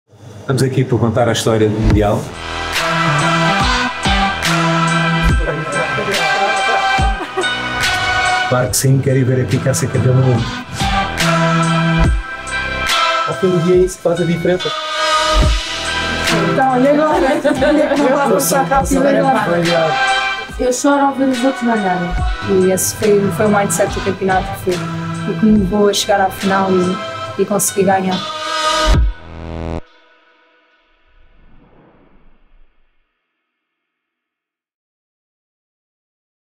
KIKA VESELKO & RODRIGO SOUSA | Club Balcão Ep.1 | Trailer
No primeiro trailer do nosso primeiro episódio, a surfista Kika Veselko e o treinador Rodrigo Sousa contam-nos como foi a experiência 'behind the scenes' de se ter sagrado campeã Mundial em San Diego, nos Estados Unidos.

Convidados ao balcão:

Gustavo Lima
Frederico Morais
Kika Nazareth
Tatiana Ferreira
Diogo Calado
Mafalda Lopes
Filipe Jervis
Teresa Abraços

Local:

Restaurante Kappo Cascais

Team:

Chef: Pedro Costa
Produção: Colure Sports
Realização: Pedro Bessa
Fotografia: Daniel Furtado